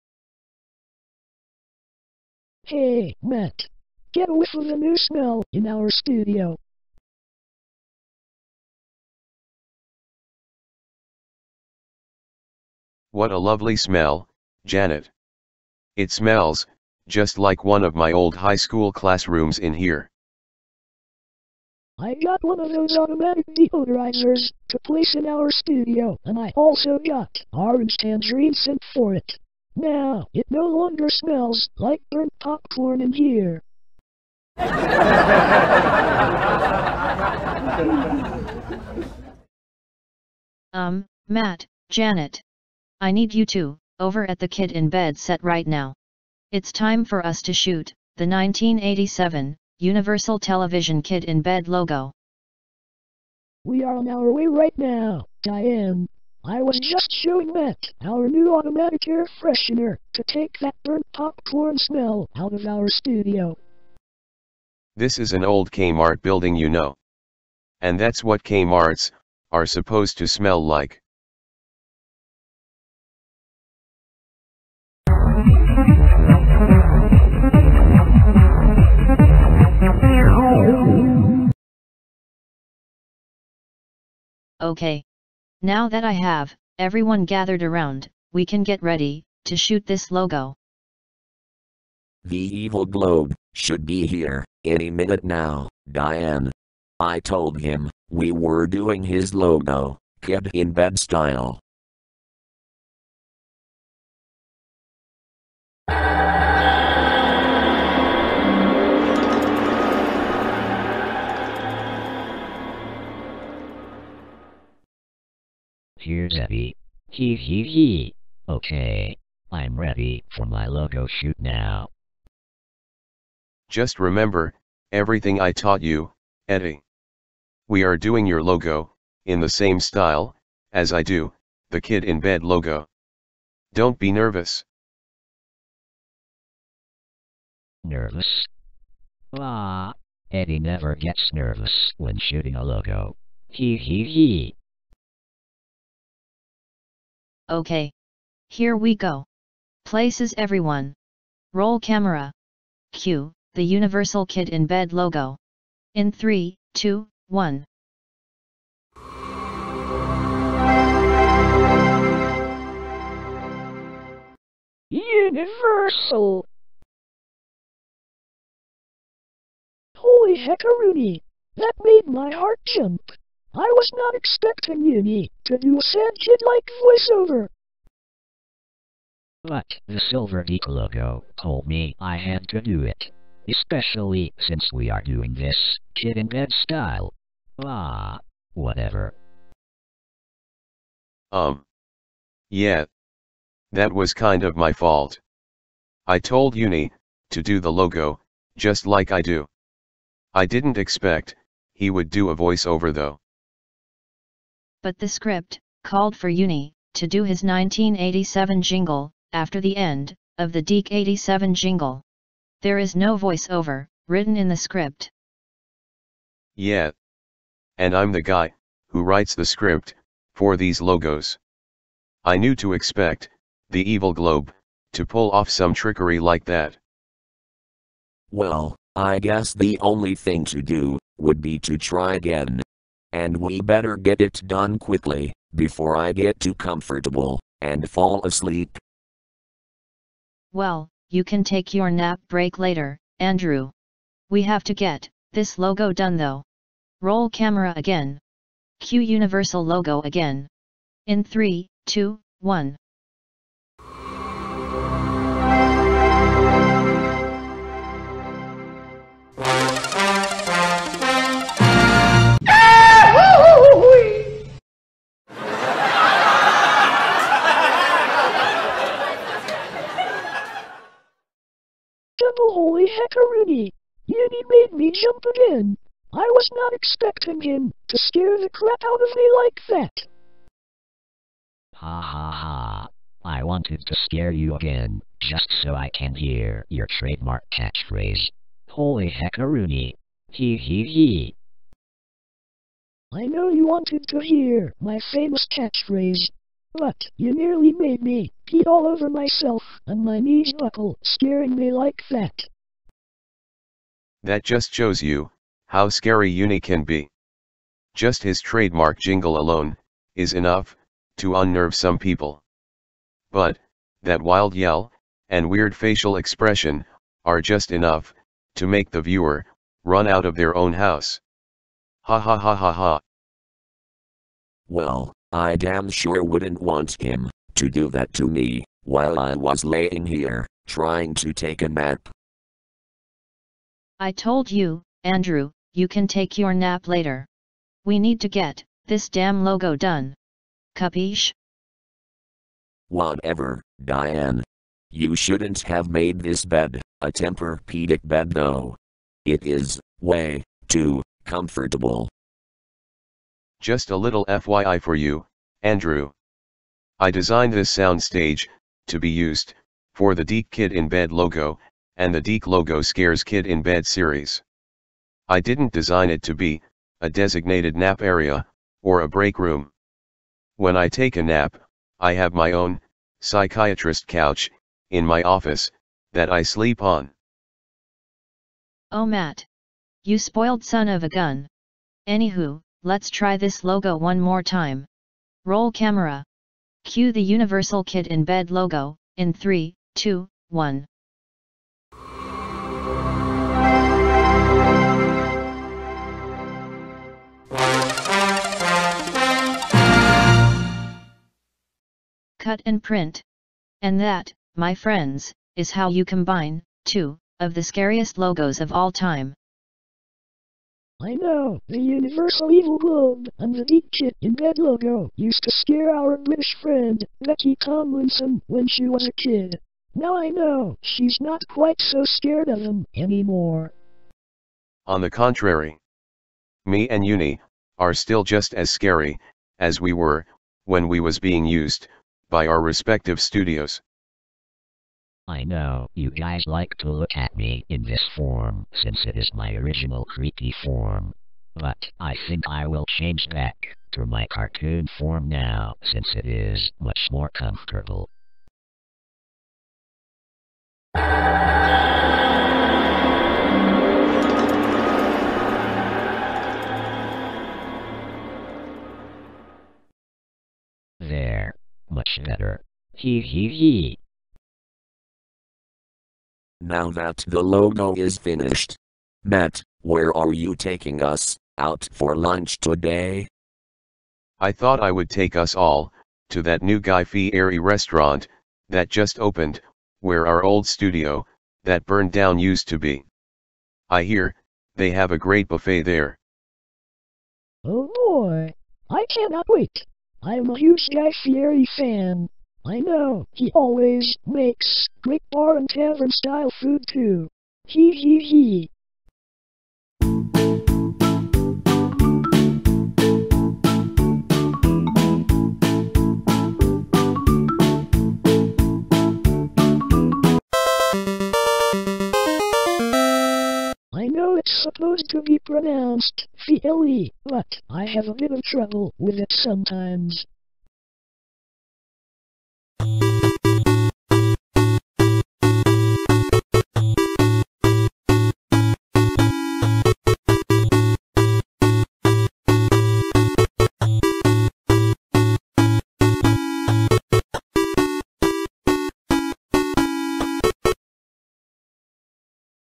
Universal Television Kid In Bed Logo Blooper (71420A)
The Evil Globe (1975 Universal Television Logo) decides he wants to do a logo in the style of the 1987 Dic Kid In Bed logo. But, since he is a bit of a prankster, things don't go necessarily as planned.

71420A